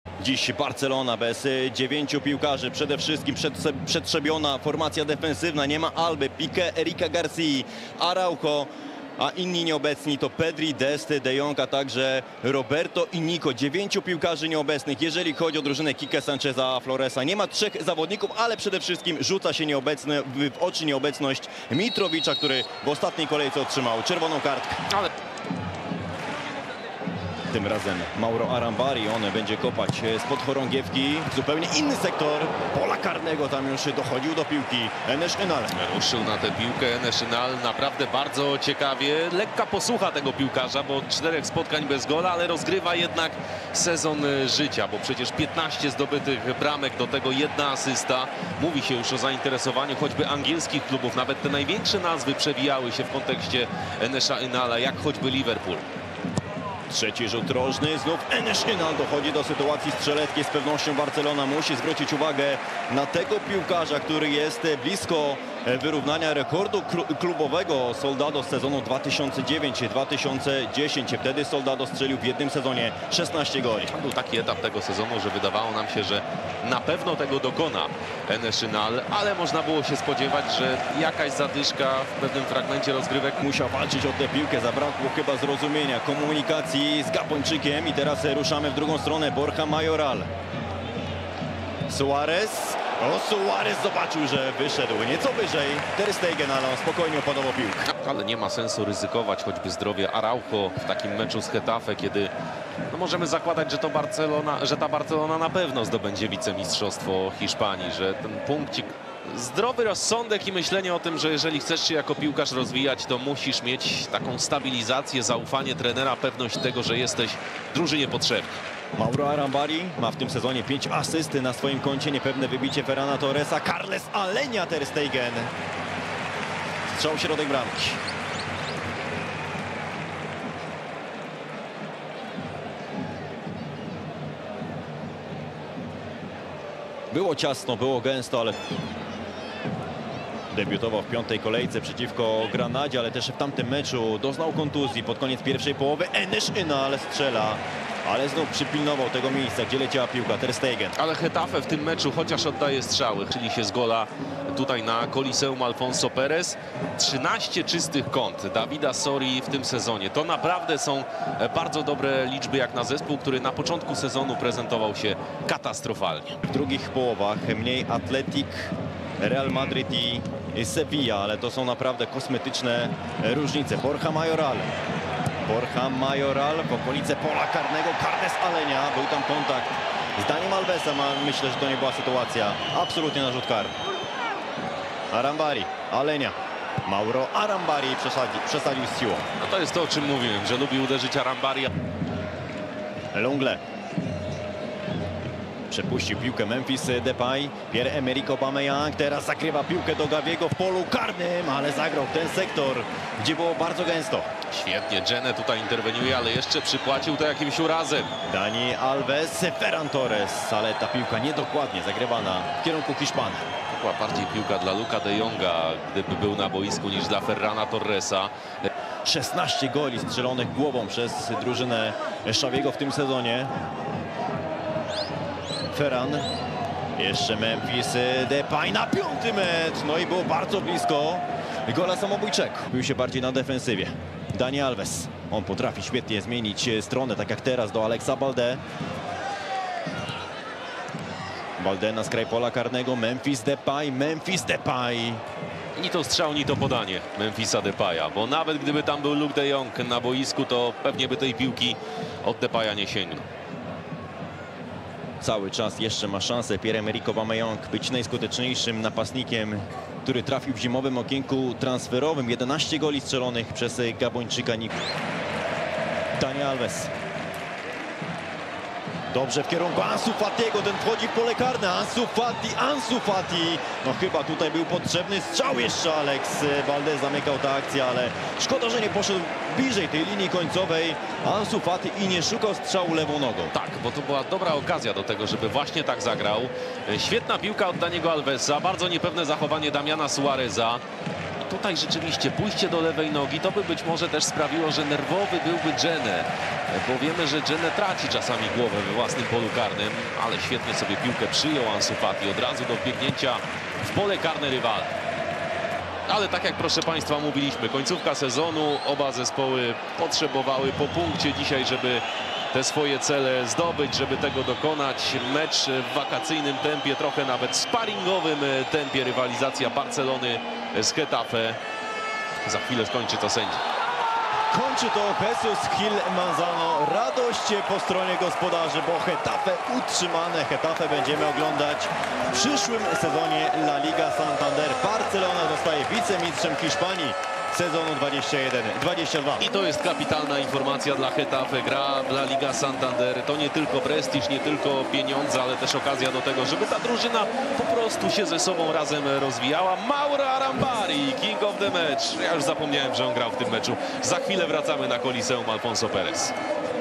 Getafe CF - FC Barcelona 15.05.2022 (skrót meczu)
FC Barcelona w 37. kolejce La Liga grała na wyjeździe z Getafe. Mecz zakończył się bezbramkowym remisem.

Eleven Sports 1
Komentarz: Michał Wszołek / Sebastian Chabiniak